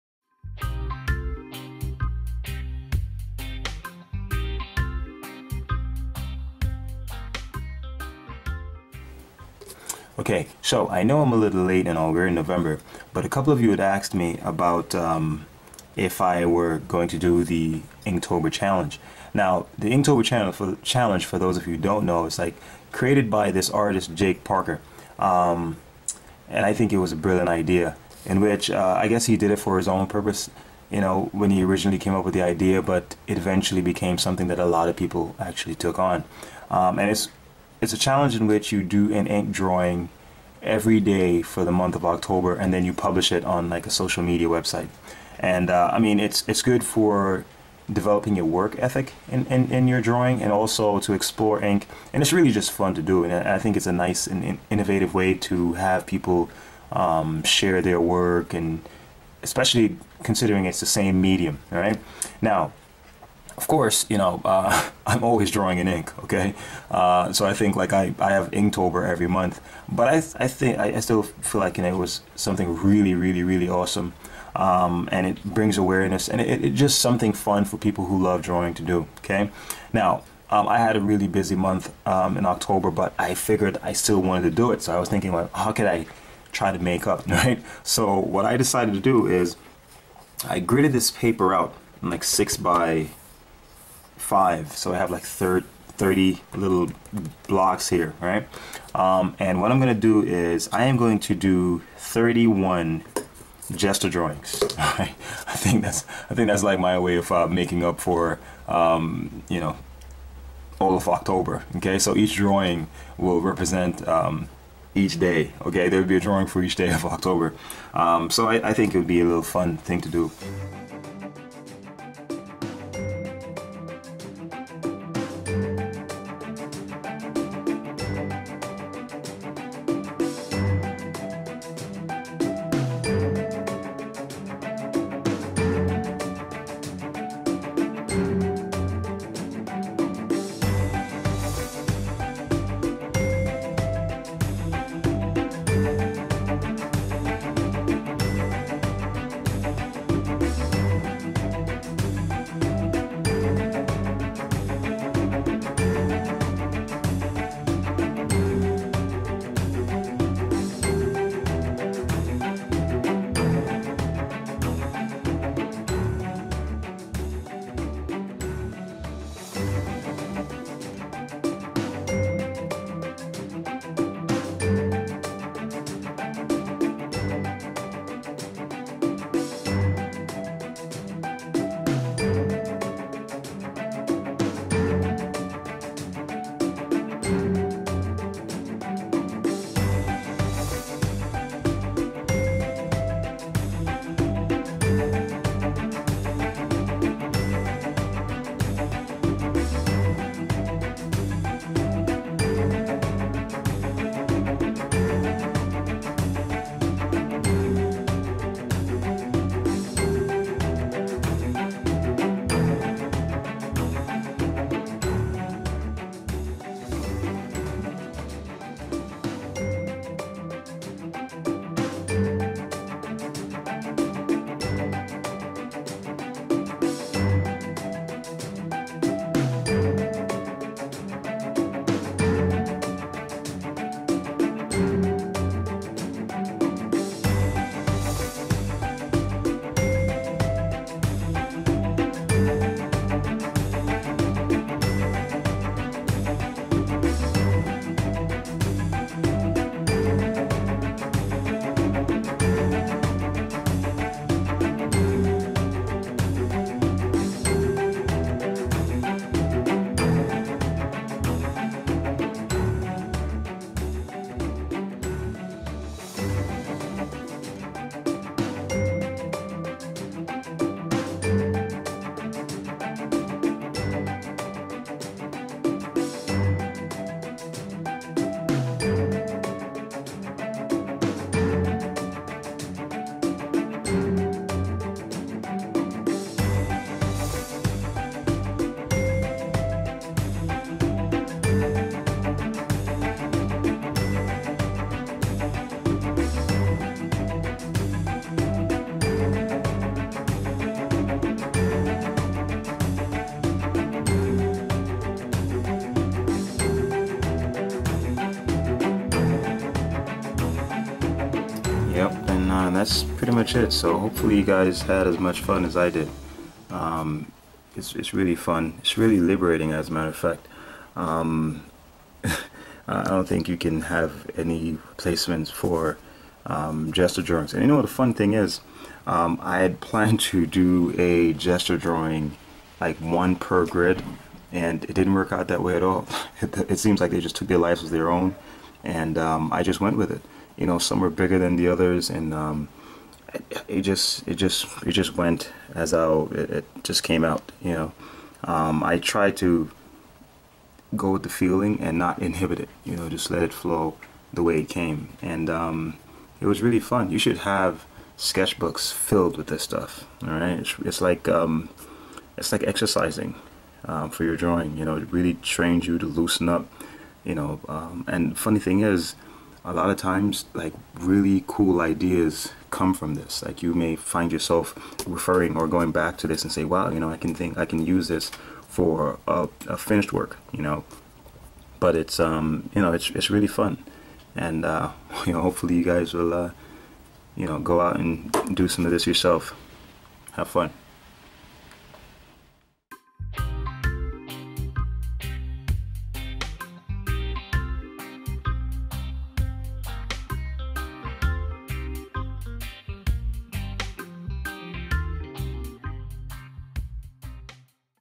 31 Quick Gesture Drawings & People Sketches
MY DRAWING BOOK
VIDEO DESCRIPTION
How to draw gestures, gesture drawing as a part of learning how to  draw and sketch people and the figure. These were quick and fun gesture drawings, each between 30 and 60 seconds.
You can do this as a warm up, for practice or just as a way to study the figure and people. Draw from life or from images you can get your hands on, but force yourself to move quickly through them. Have fun!!